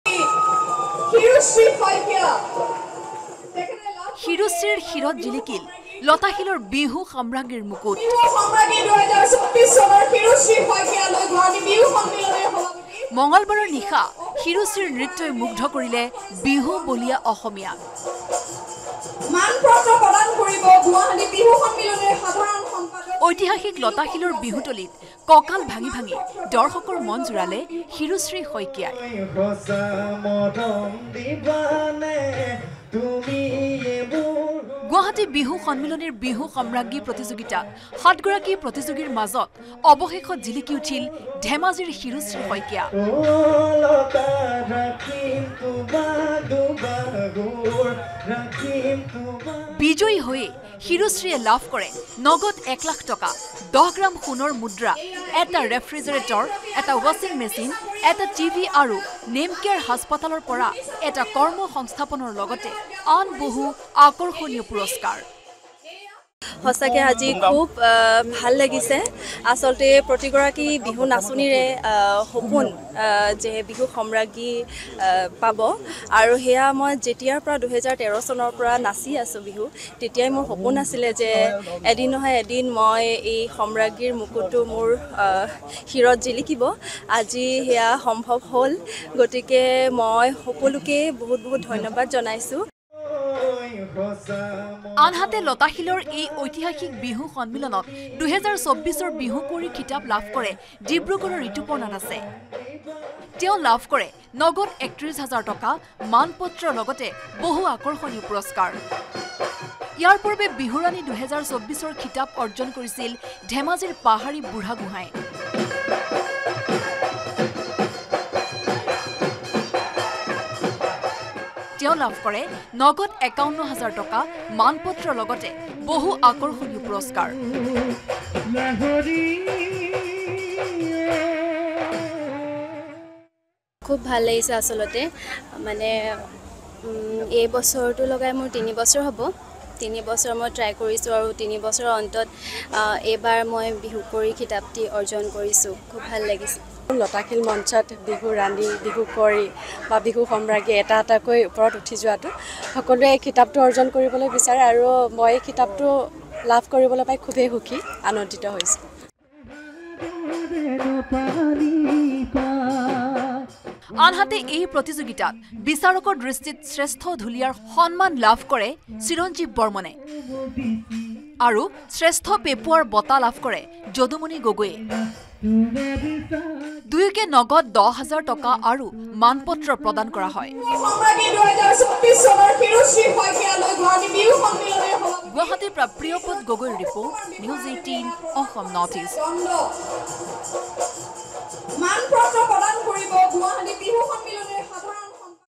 Bihu Samragi 2024 | শিৰোশ্ৰী শইকীয়াৰ শিৰত জিলিকিল গুৱাহাটী বিহু সন্মিলনীৰ বিহু সম্ৰাজ্ঞী মুকুট N18V
Bihu Samragi winner | গুৱাহাটী বিহু সন্মিলনীৰ বিহু সম্ৰাজ্ঞী প্ৰতিযোগিতাত ৭ গৰাকী প্ৰতিযোগীৰ ভিতৰত ধেমাজিৰ শিৰোশ্ৰী শইকীয়াই লাভ কৰিলে এই খিতাপ।

লগতে চাওঁক দিনটোৰ বহু গুৰুত্বপূৰ্ণ খবৰ |